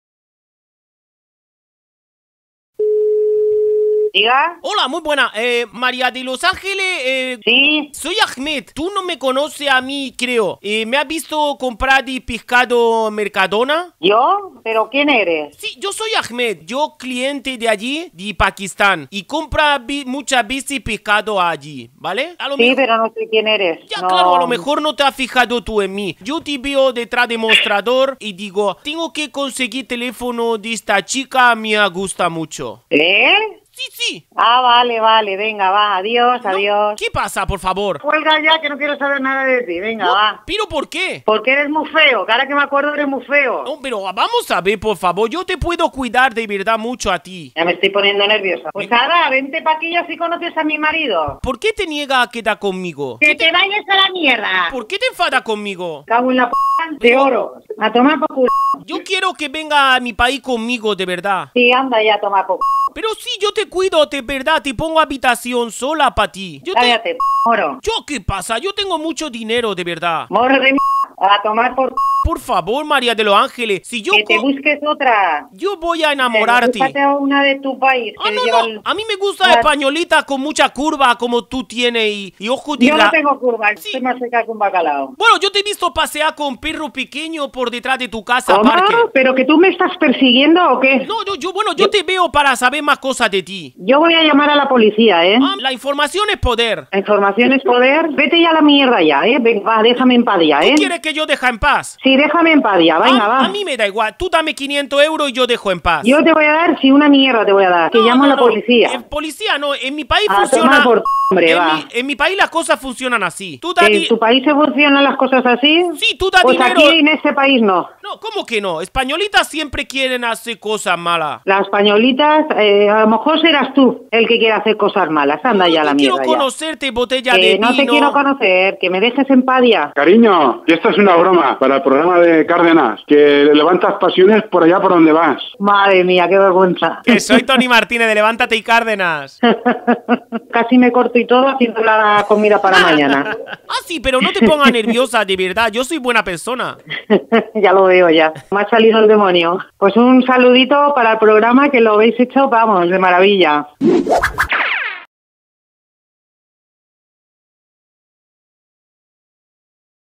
TonyMartinez.es - Ahmed se insinúa a pescadera del Mercadona y acaba queriendo extorcionarla
Ahmed se insinúa a pescadera del Mercadona y acaba queriendo extorcionarla